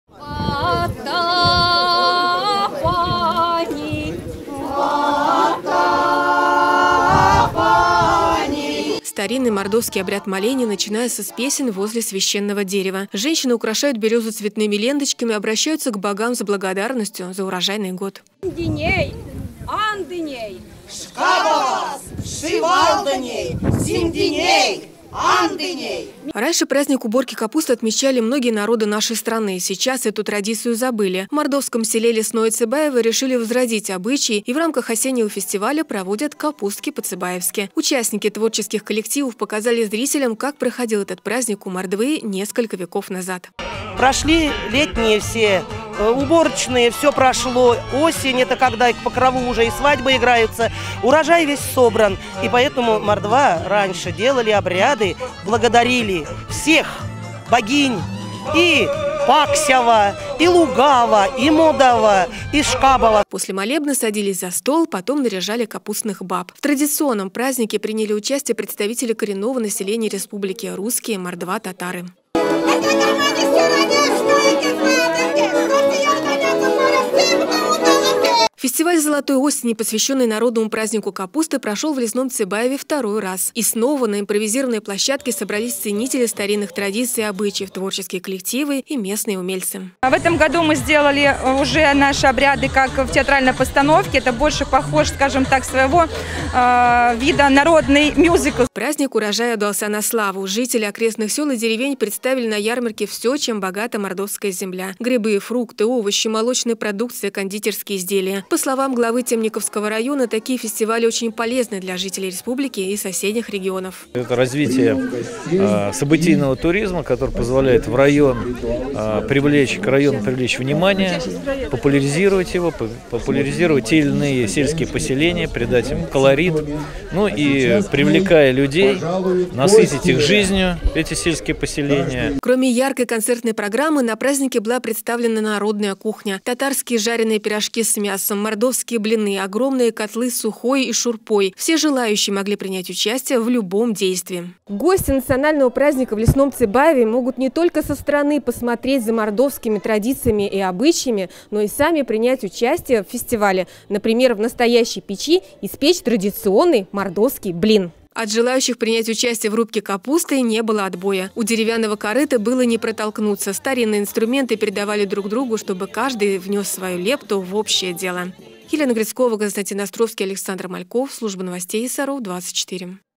Капустки по-Цибаевски
В селе Лесное Цибаево Темниковского района прошёл II фестиваль золотой осени «Капустки по-Цибаевски».